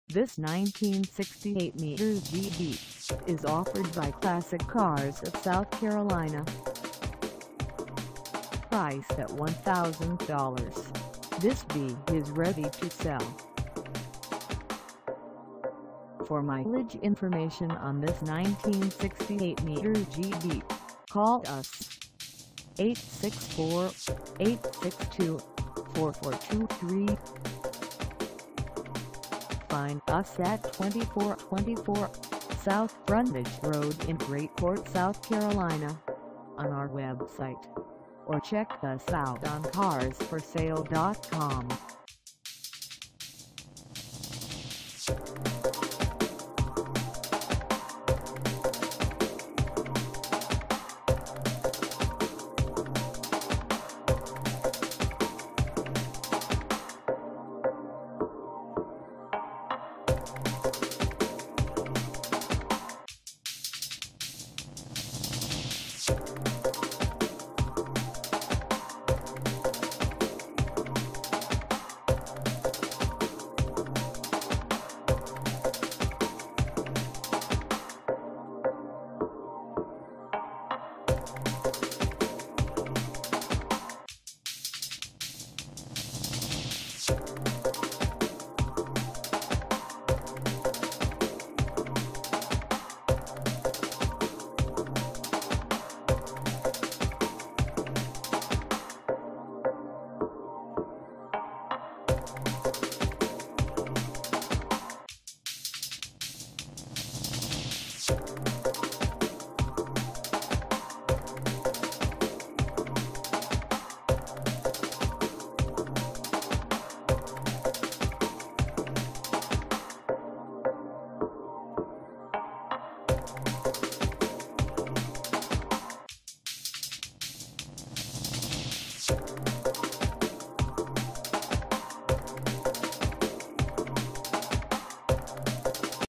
1968 MG B for sale in Gray Court, SC 29645 at the Classic Ca
This 1968 MG B is for sale in Gray Court, SC 29645 at Classic Cars of South Carolina.
Engine:
Transmission:
Mileage: 0
Contact Classic Cars of South Carolina 2424 South Frontage Rd Gray Court, SC 29645 to test drive this 1968 MG B  today.